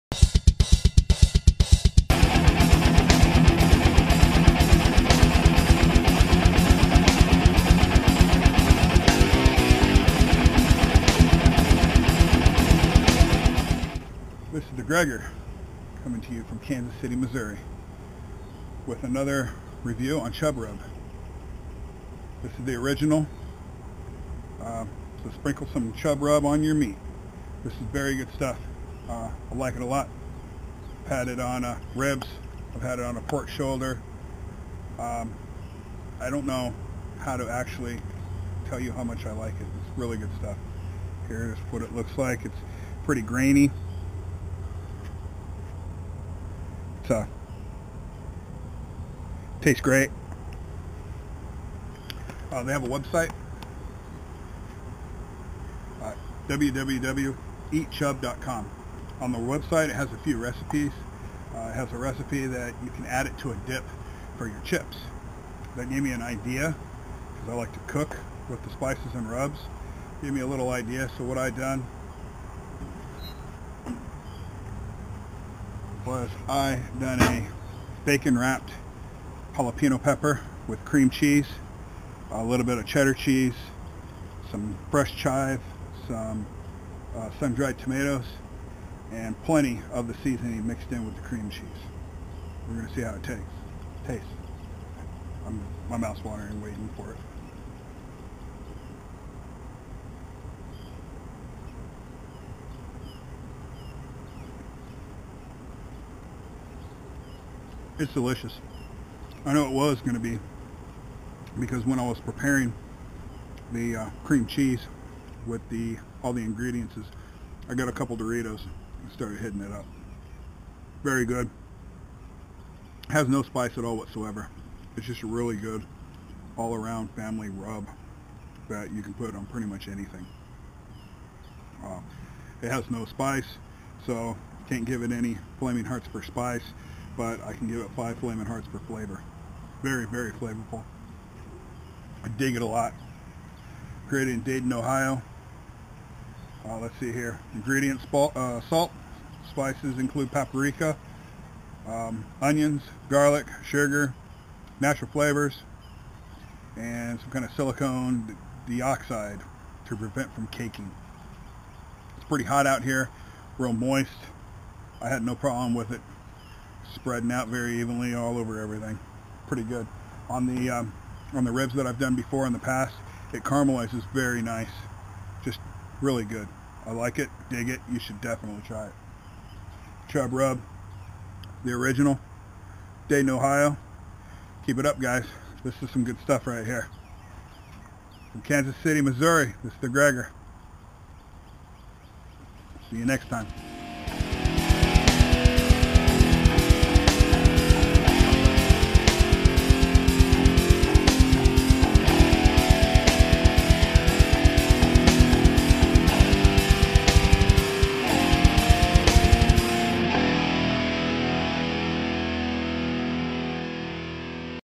CHUB RUB ORIGINAL
The Gregger reviews Chub Rub Original for ILOVEITSPICY.COM